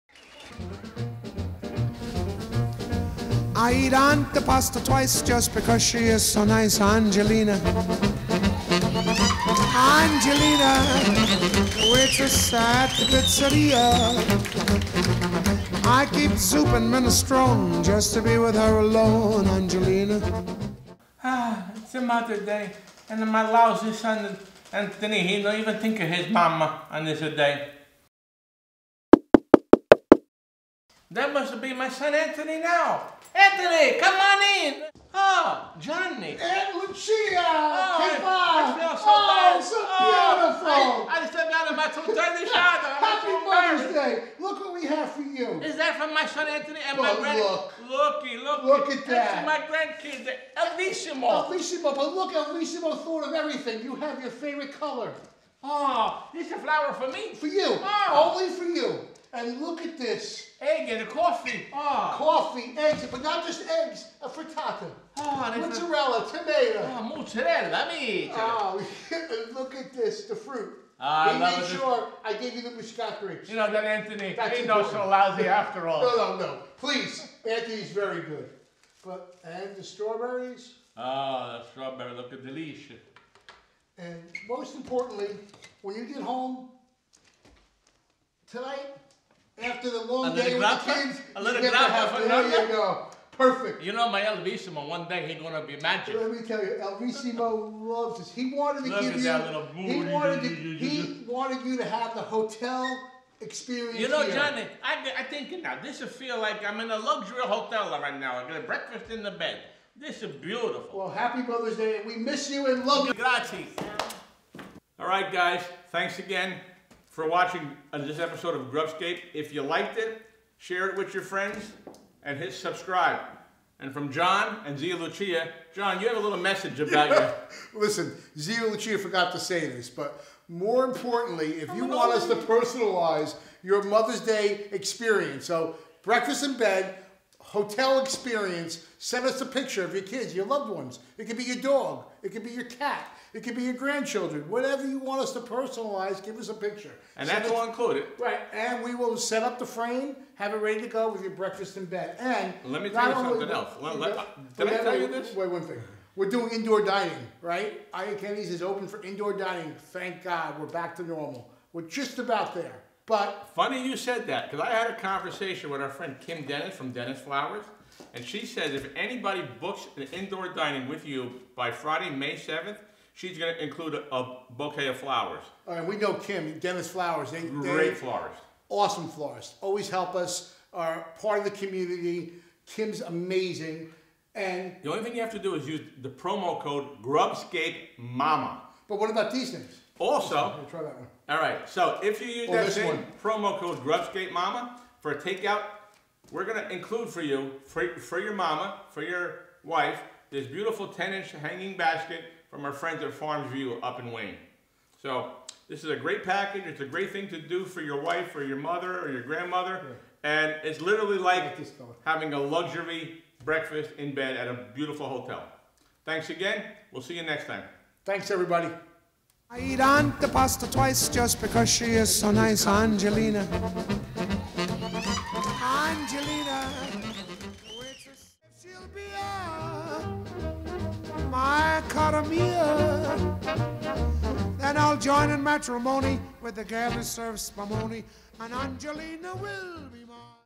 GrubScape Special: Mamma!
Zia Lucia, che bella! Shoutout to our "guest star" in today's episode of GrubScape. 💐

Iachetti's  is offering "Breakfast in Bed" and "Brunch Box" menus that will bring the luxury room service experience right to your front door. If you place your order before Friday 5/7, we will also print a personalized photo of your choice AND throw in a fresh bouquet from Denis Flowers to add the special touch to your Mother's Day Brunch.

Thank you as always for the support, and be sure to get those Mother's Day orders in before 5/7 to receive these incredible free gifts!

📹 Bill Shaw